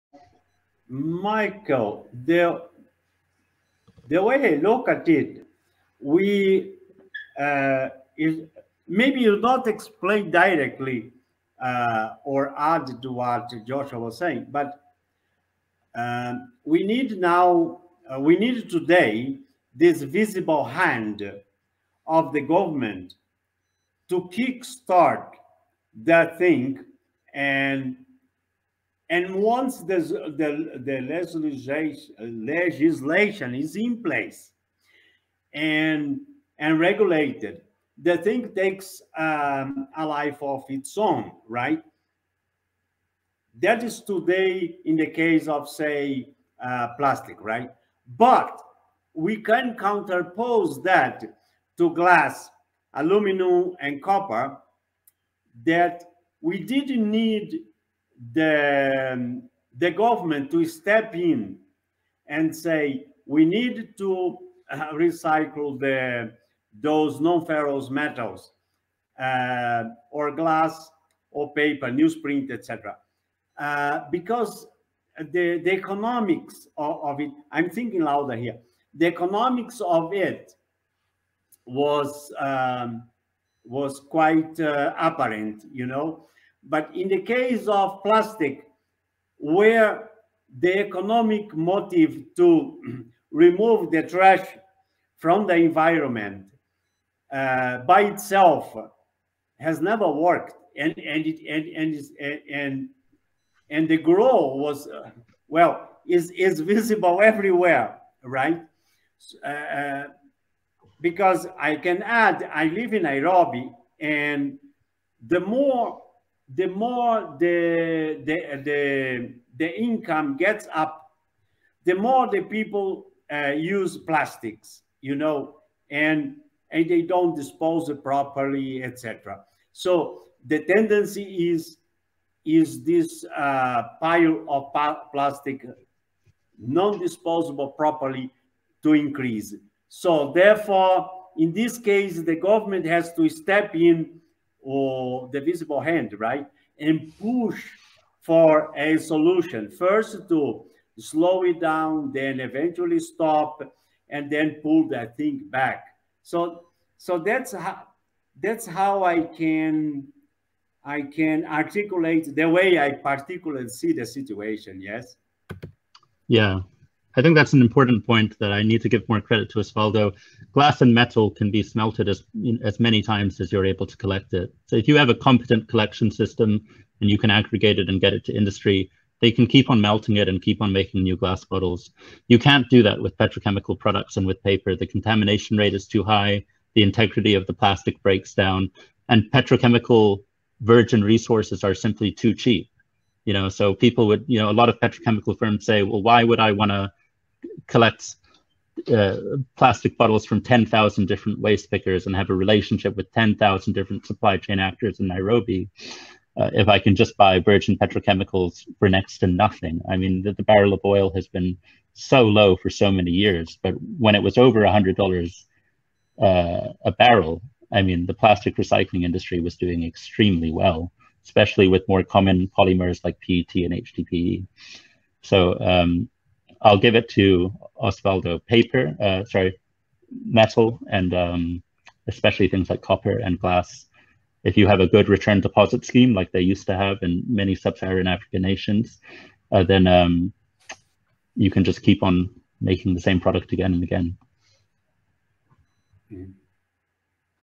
Osvaldo Coelho, Joshua Palfreman and the Invisible Hand
Osvaldo Coehlo and Joshua Palfreman discuss why recycling works for certain minerals and metals without government encouragement and not for others